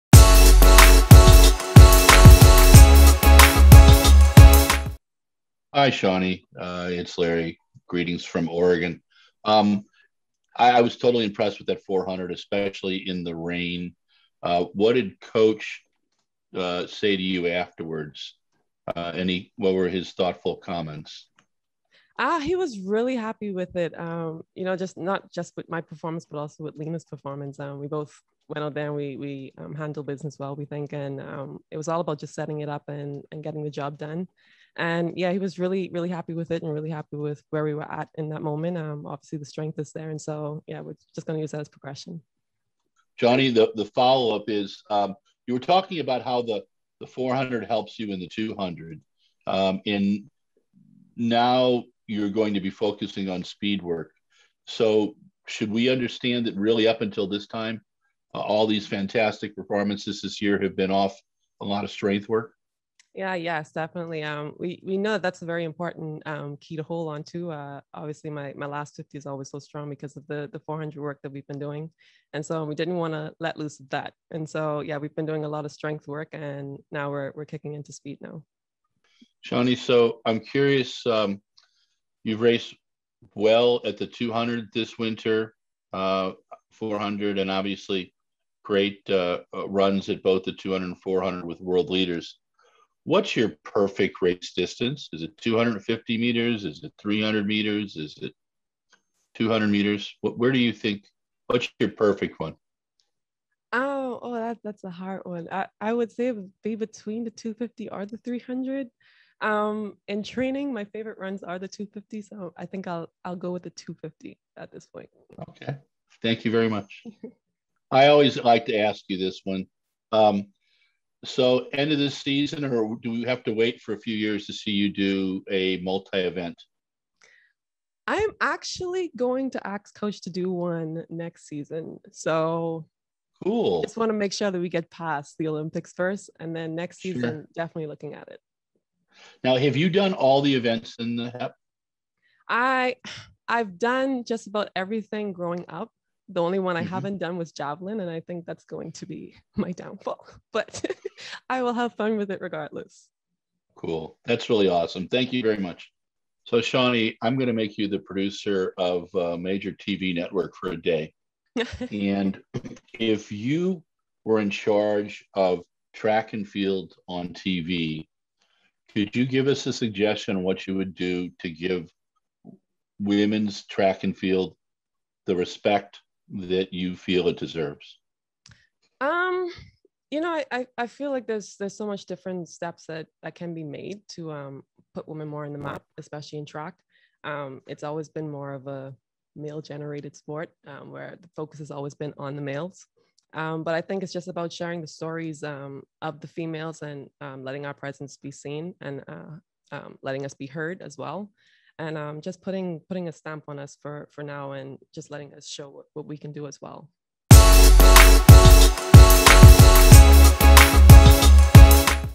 Shaunae Miller-Uibo - adidas Boost Boston Games 2021
RunBlogRun presents
Shaunae Miller-Uibo
adidas Boost Boston Games 2021
Press Conference